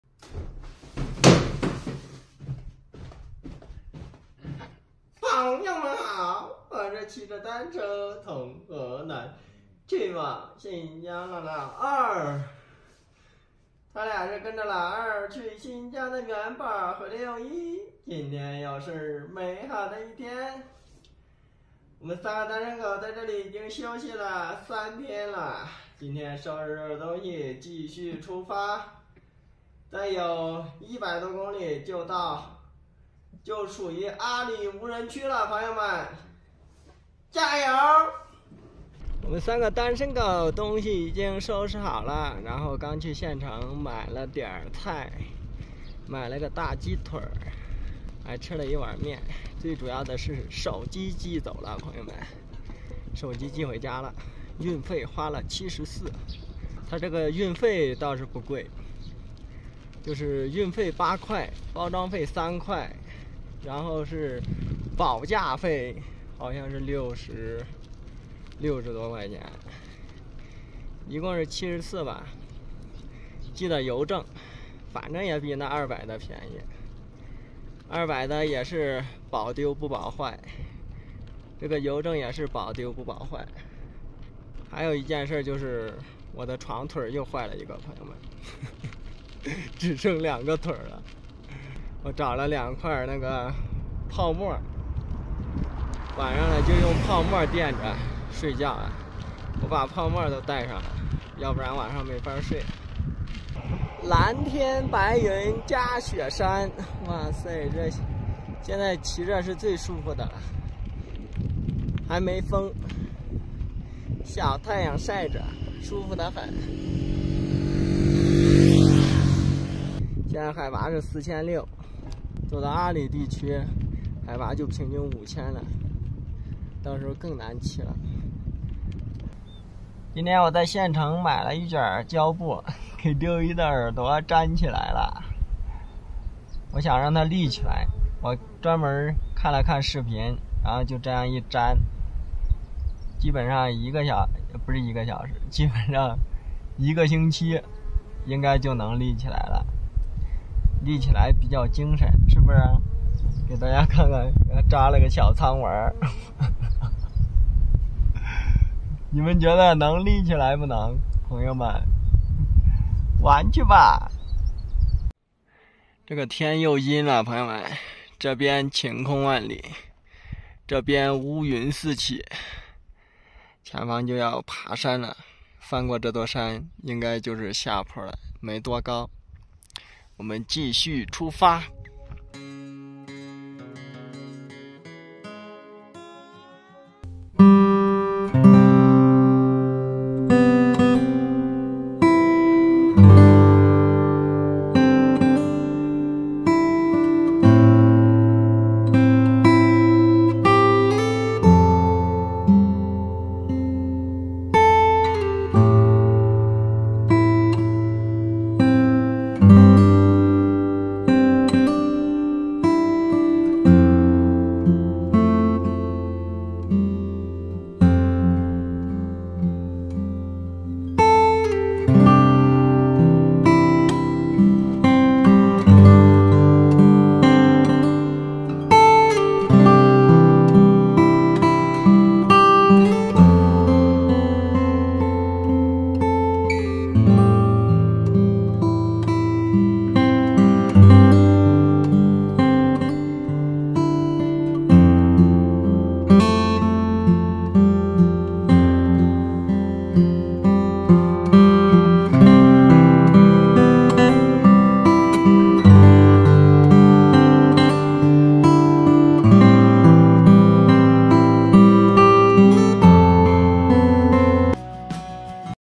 小伙穷游旅行410天虽然越来越像流浪汉，但是很自由
朋友们好
我是骑着单车从河南去往新疆了
老二
他俩是跟着老二去新疆的
元宝和六一今天又是美好的一天
我们三个单身狗在这里已经休息了
三天了今天收拾收拾东西继续出发
再有100多公里就到
就属于阿里无人区了朋友们
加油
我们三个单身狗东西已经收拾好了
然后刚去县城买了点菜
买了个大鸡腿
还吃了一碗面
最主要的是手机寄走了
朋友们手机寄回家了
运费花了74
他这个运费倒是不贵
就是运费8块包装费3块
然后是保价费好像是6060多块钱
一共是74吧
寄的邮政
反正也比那200的便宜
2百的也是保丢不保坏
这个邮政也是保丢不保坏
还有一件事就是我的床腿又坏了一个
朋友们
只剩两个腿了
我找了两块那个泡沫
晚上呢就用泡沫垫着睡觉啊
我把泡沫都带上了
要不然晚上没法睡
蓝天白云夹雪山
哇塞这现在骑着是最舒服的了
还没风
小太阳晒着舒服的很
现在海拔是4,600
走到阿里地区海拔就平均5,000了
到时候更难骑了
今天我在县城买了一卷胶布
给六一的耳朵粘起来了
我想让他立起来
我专门看了看视频
然后就这样一粘
基本上一个小不是一个小时
基本上一个星期应该就能立起来了
立起来比较精神是不是
给大家看看扎了个小苍丸
哈哈哈
你们觉得能立起来不能
朋友们
玩去吧
这个天又阴了
朋友们这边晴空万里
这边乌云四起
前方就要
爬山了翻过这座山应该就是下坡了
没多高
我们继续出发